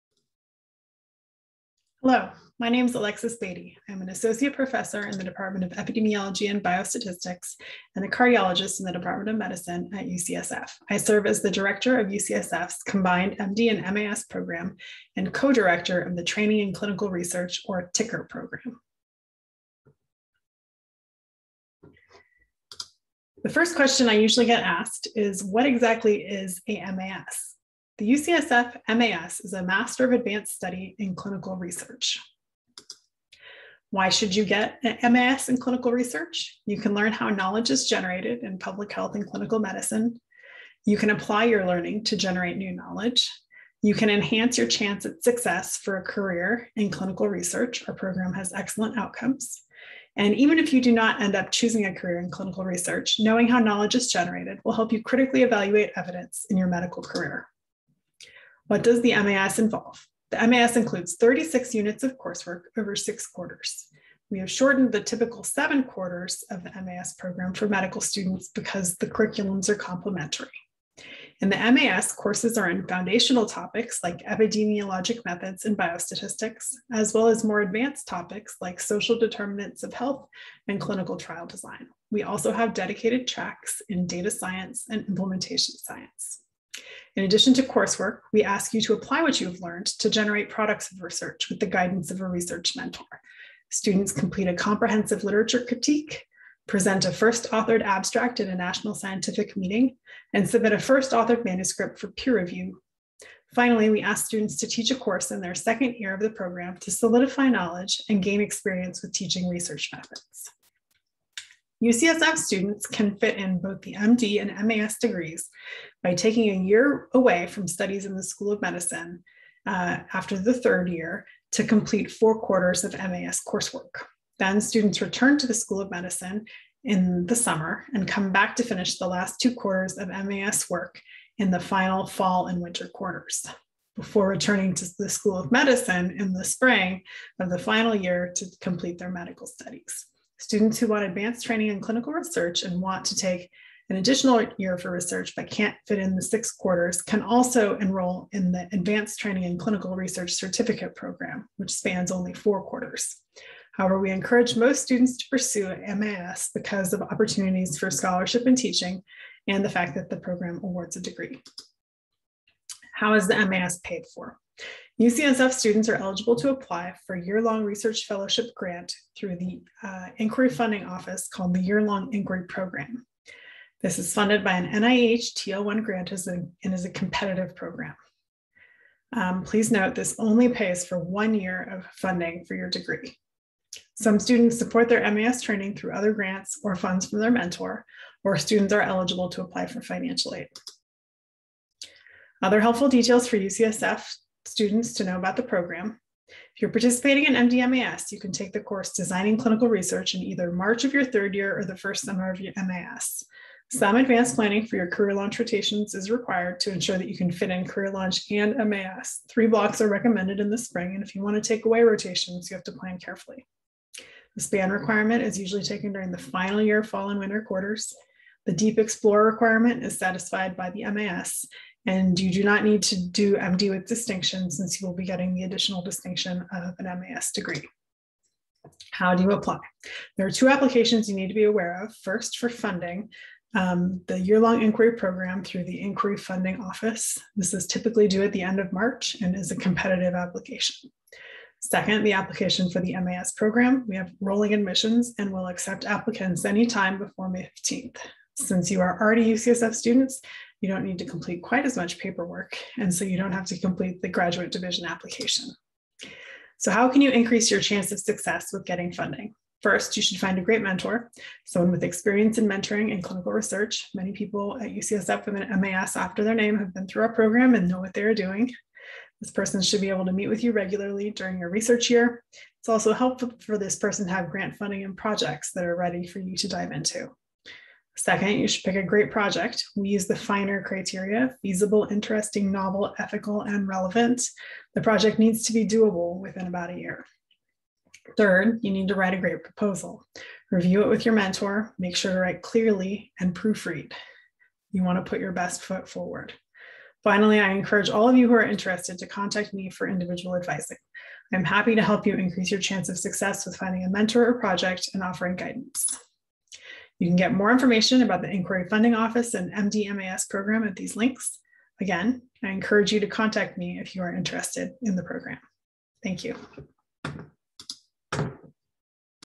An overview of the MD/MAS degree in Clinical Research at UCSF (2021 update)
Program Director Alexis Beatty walks prospective students through an overview of the MD/Master in Advanced Studies (MAS) degree in Clinical Research at UCSF.

Medical students interested in receiving rigorous training in the methods and conduct of clinical research can obtain a Master in Advanced Studies (MAS) degree in clinical research in addition to an MD degree in a 5-year combined MD/MAS program. MAS coursework includes instruction in advanced epidemiologic and biostatistical methods with electives in specialized topics such as causal inference, prediction science, molecular methods in clinical research, implementation science, and decision and cost-effectiveness analysis. Students graduating from the program will be poised prepared for careers as leaders in clinical research and medicine. Learn more about the MD/MAS program. See a model of the 5-year academic calendar. Contact the MD/MAS program director.